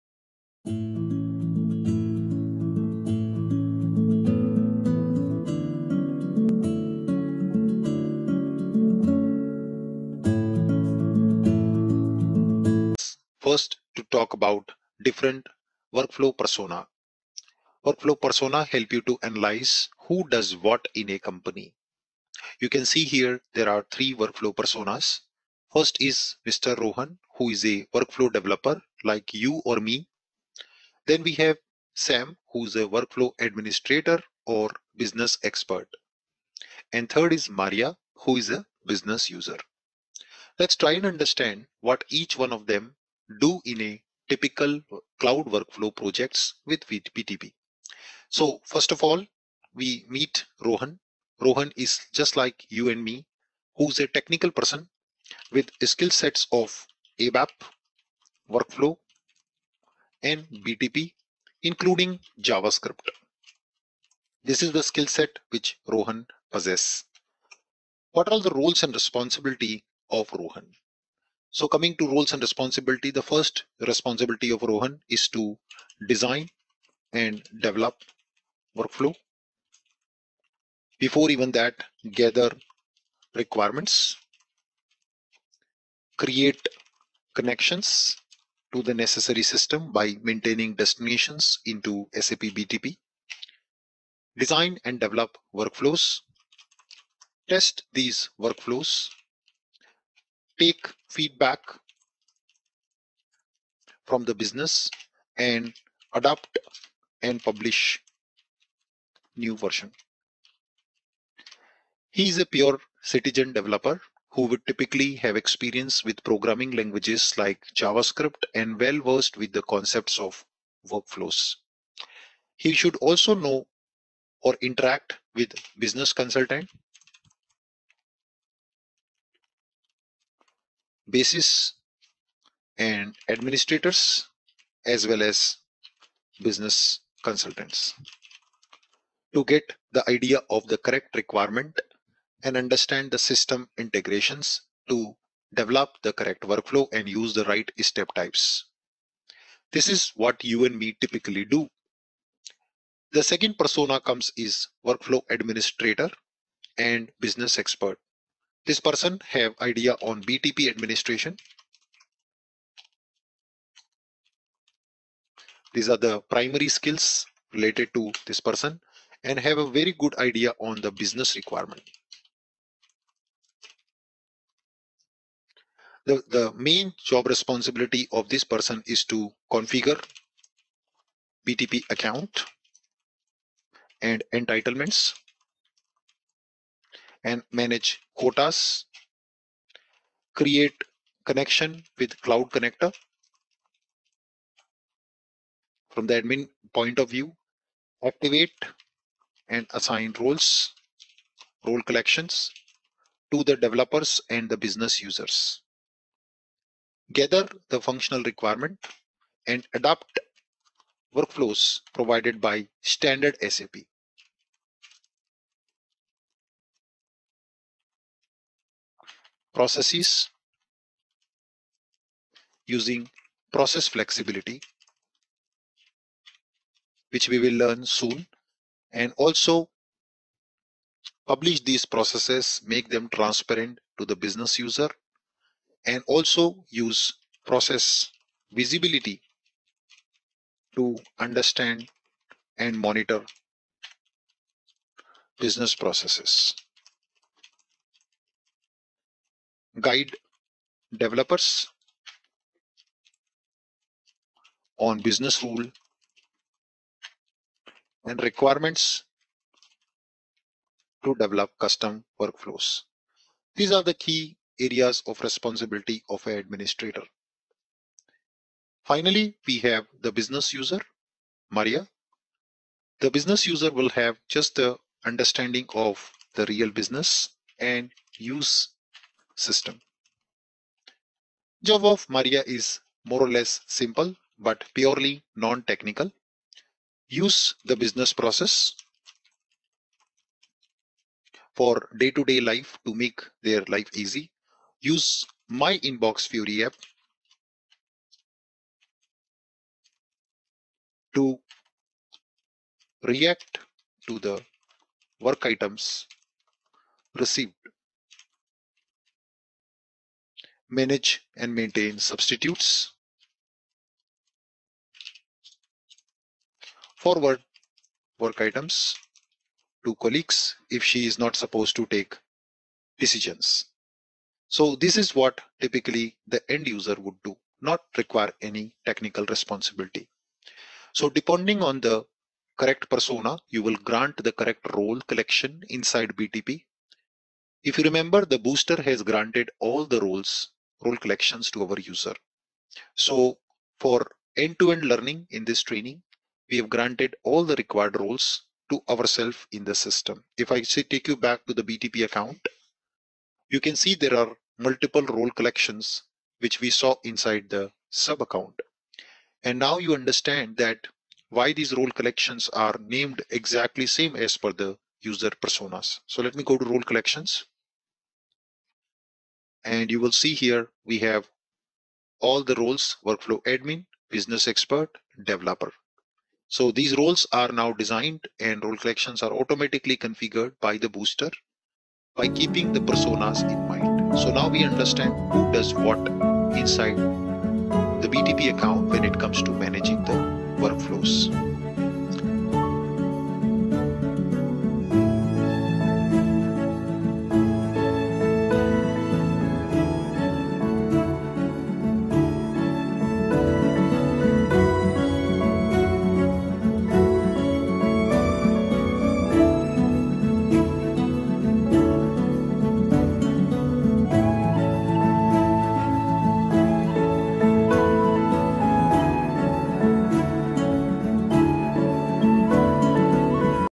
SAP BTP Workflow Developer Persona | Workflow in the Cloud Foundry Environment | BTP Workflow in BAS
Learn to build a basic workflow in the Cloud Foundry environment of SAP Business Technology Platform by adding a user task to your workflow that you then display and process in the SAP Fiori-based My Inbox, and creating a start form for end users to start and complete the workflow application. (For an end-to-end experience of Workflow Management – using the workflow, business rules, and visibility capabilities – see this video.